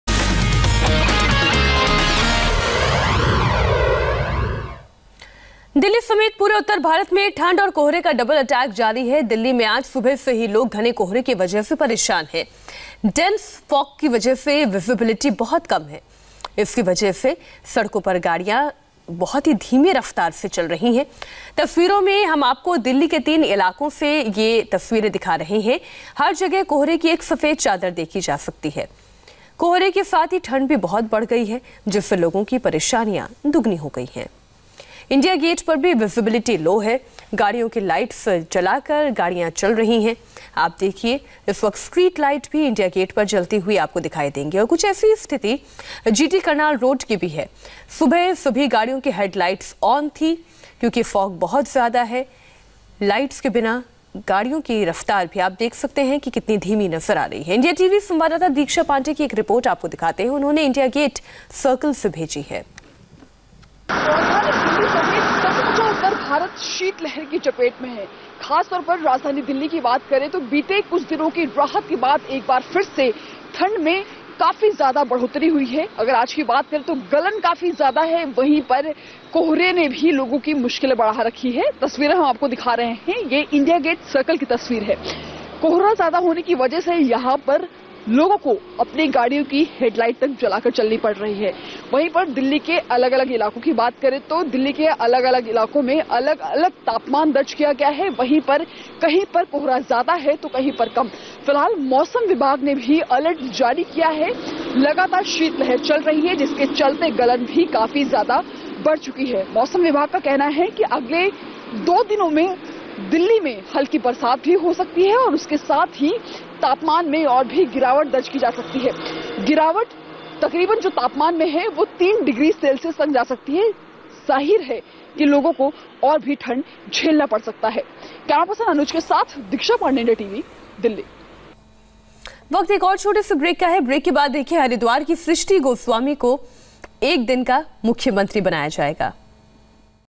Dense Fog Shrouds Delhi, Mercury Drops Further; AQI In 'Very Poor' Category | IndiaTV News
Parts of India witnessed dense fog on Friday, and cold weather conditions are likely to prevail at several places.

The national capital remained enveloped in a layer of fog, as the temperature dropped in the city. The current temperature in the national capital is 6.8° Celsius, said the India Meteorological Department (IMD) on Friday morning.